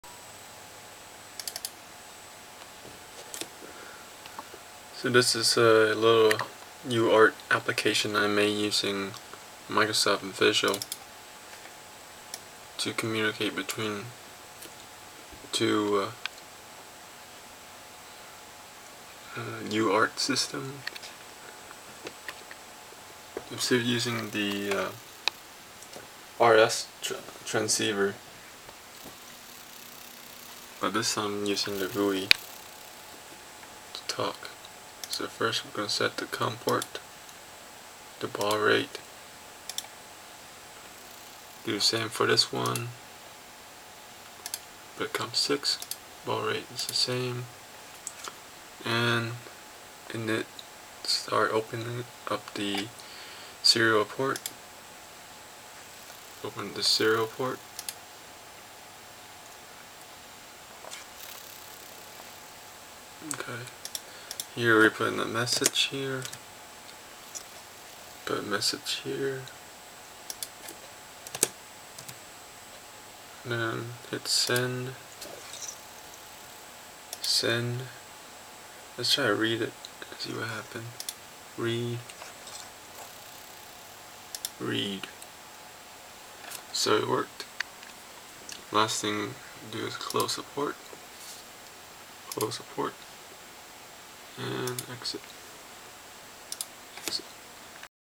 visual c++ and serial port for embedded uart
Demonstration of using visual c++ windows forms to create gui for
uart serial communication.
I also used two rf transceivers in this demo
Note: you will need a usb to ttl cable

for sample code